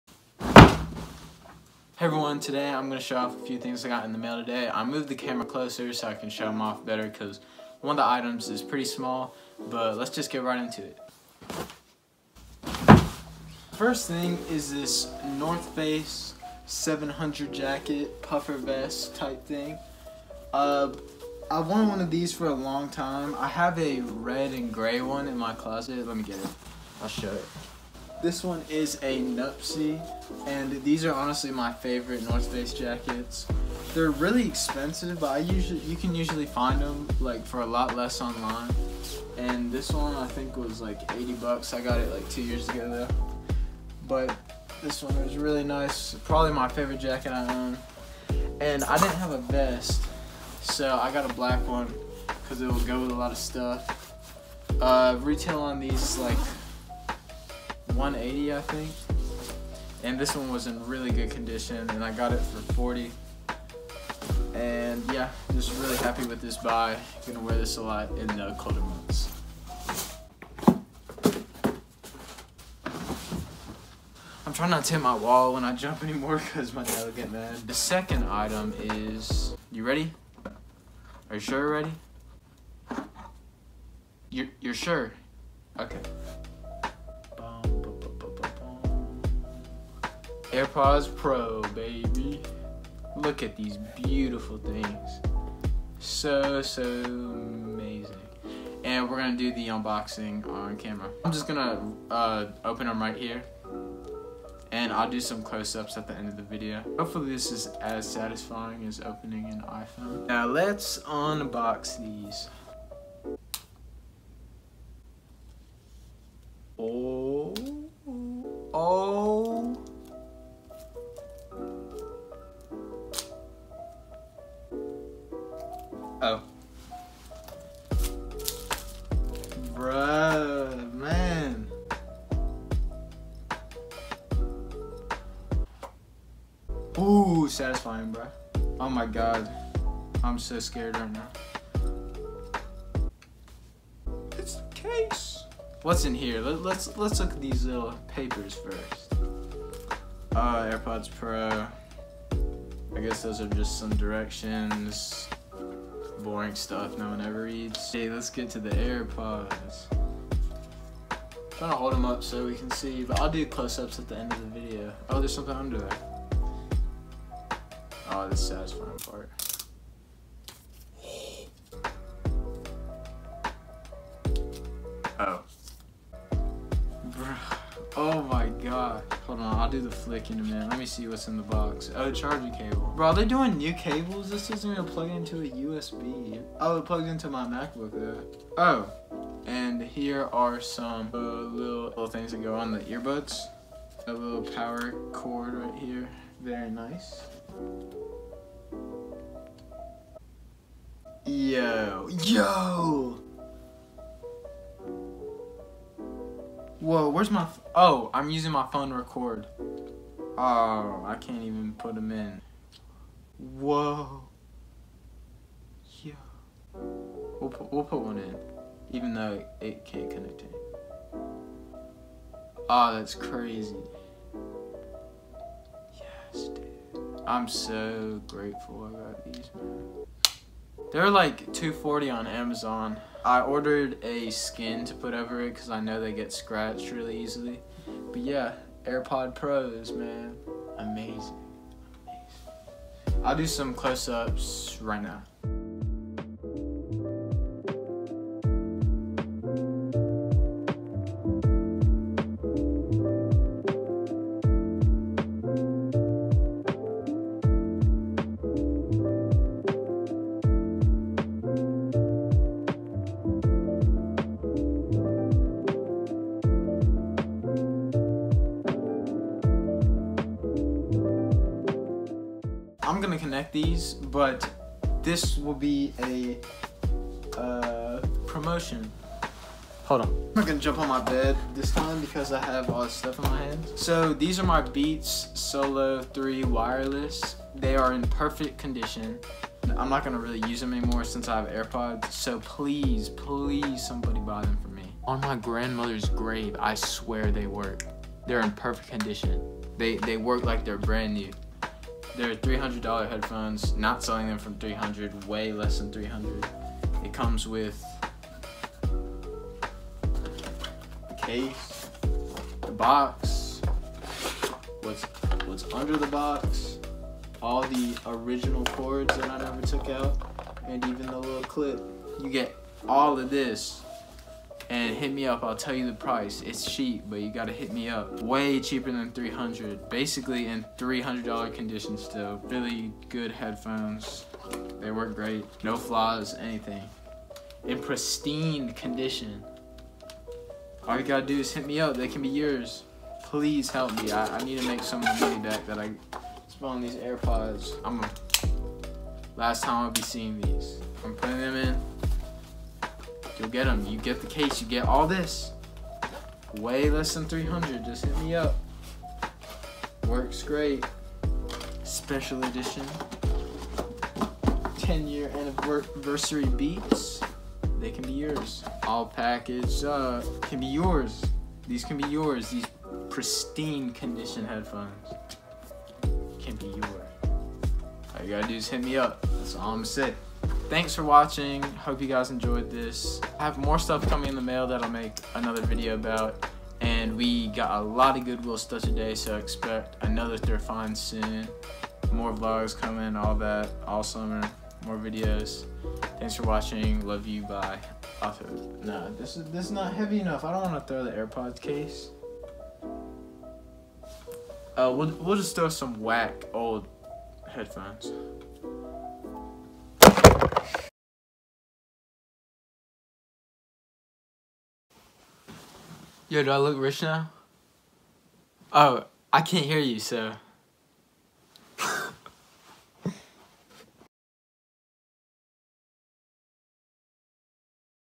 AIRPOD PRO UNBOXING (Unboxing video)
I unbox airpods and a jacket. Please buy my beats.
Selling page on instagram: willsgoodfinds
Grailed: wbendall01
Depop: wbendall01